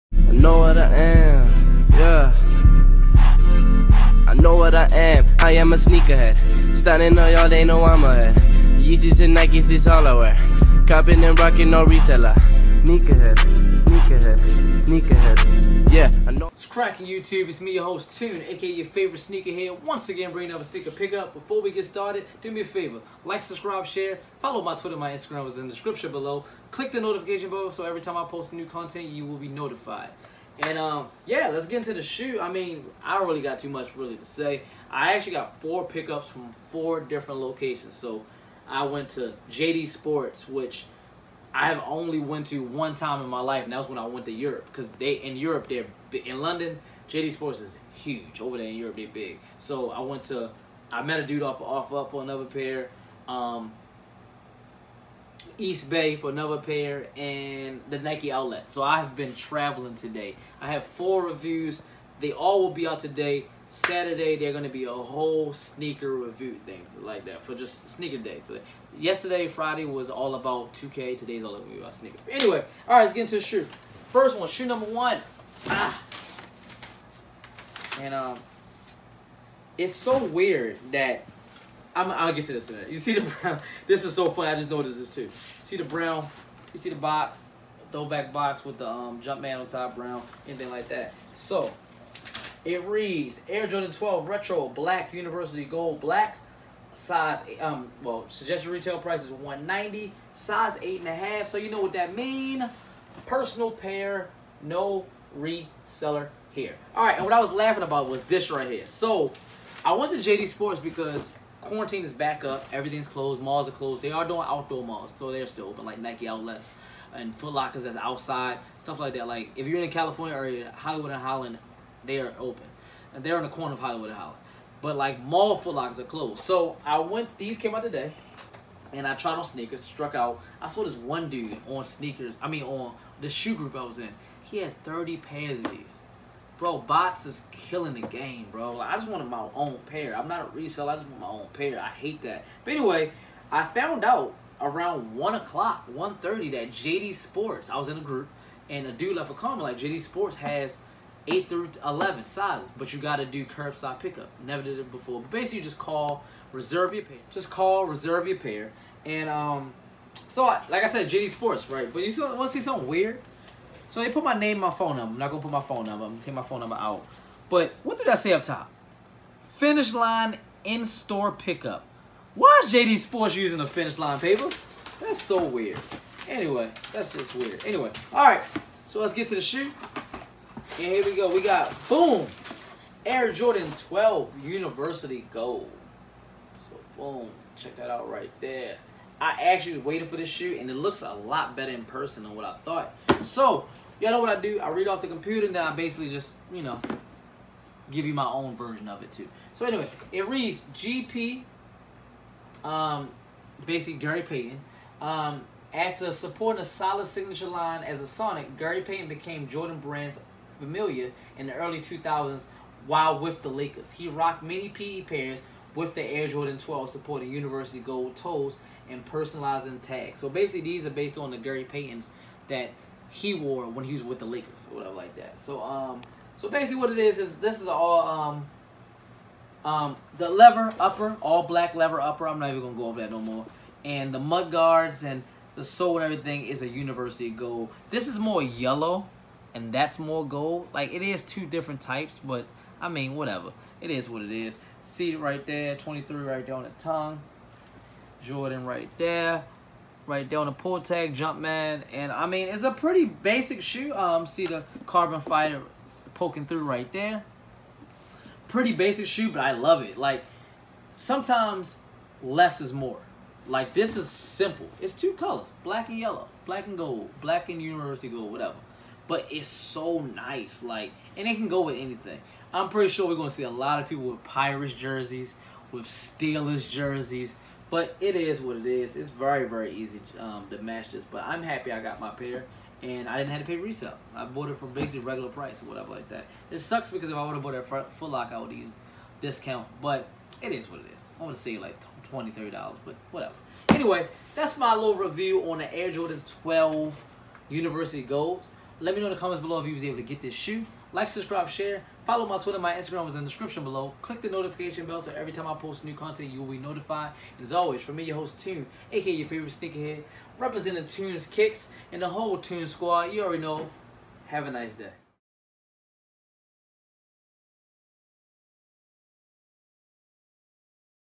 Air Jordan 12 "University Gold” Sneaker Review
Follow My Sneaker Instagram @Toons_Kiccs
Follow my Sneaker Twitter @TKiccs

New Air Jordan 12 “University GOld” Sneaker Review